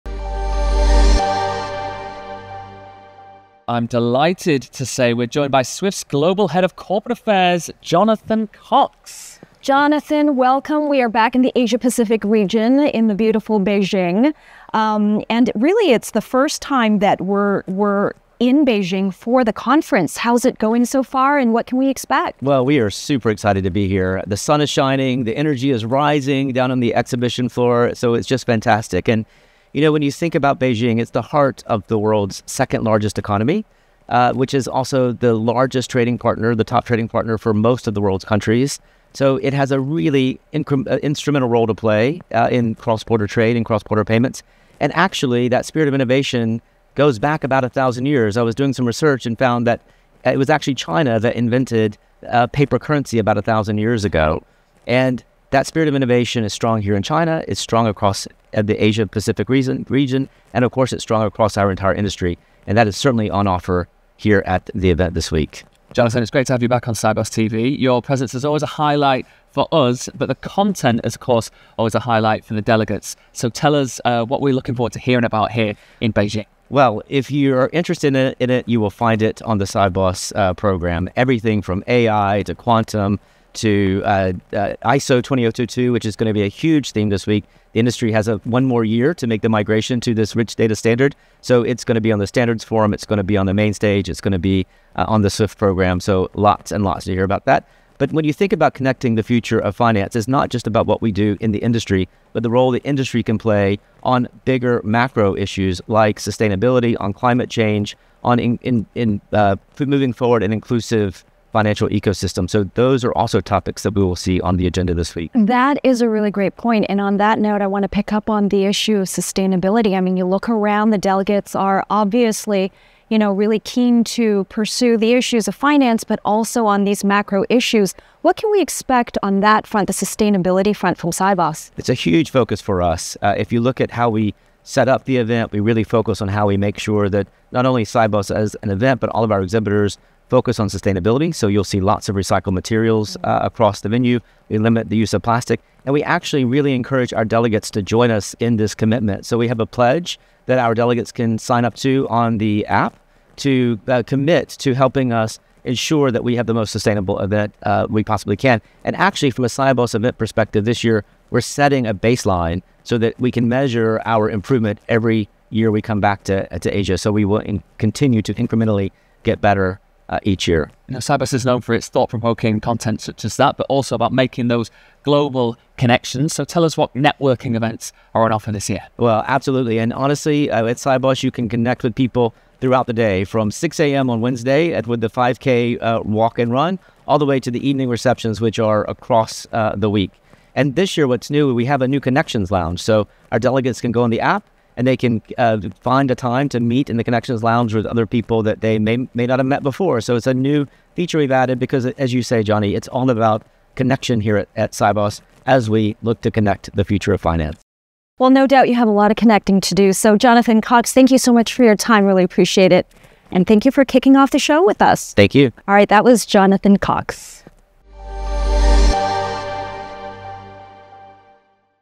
Sibos TV: Welcome to Sibos 2024 – October 2024
Swift's Managing Director of Corporate Affairs, Jonathan Cox, speaks to Sibos TV’s Angie Lau and Jonny Nelson about Sibos returning to Asia Pacific and the event's first time in Beijing.